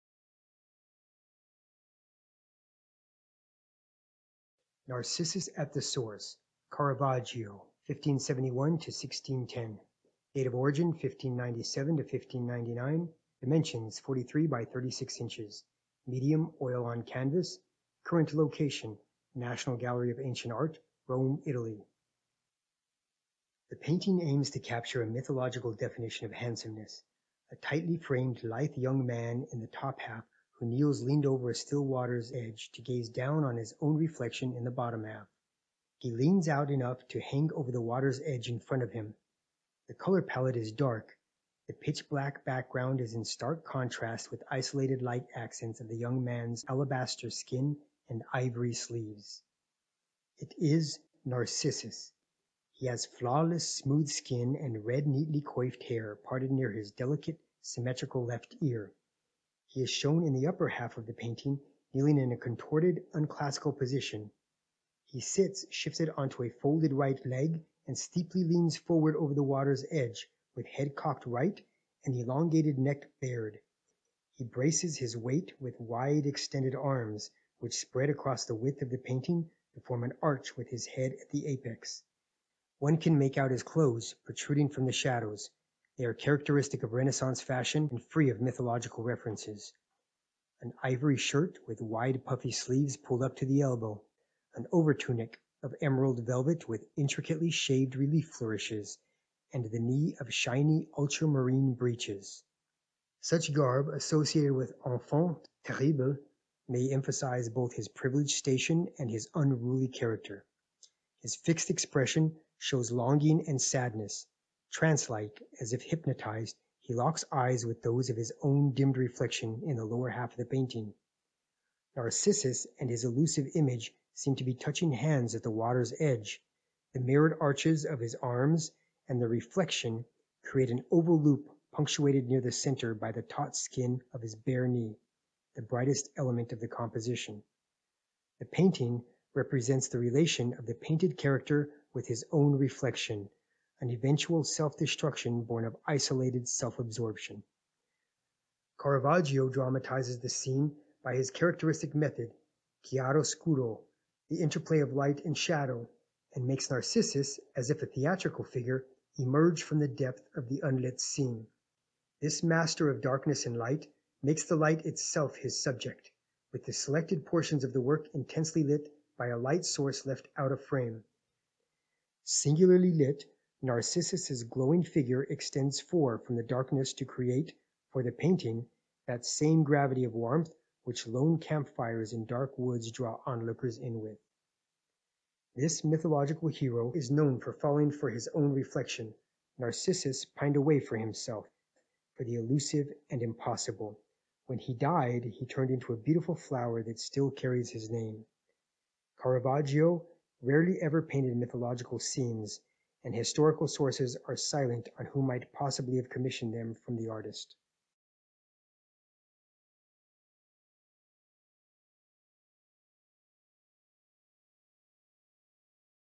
"Narcissus at the Source," "Speaks the Masterpiece" advance audio description
For Advance Readers: a sample audio-description of Caravaggio's "Narcissus at the Source."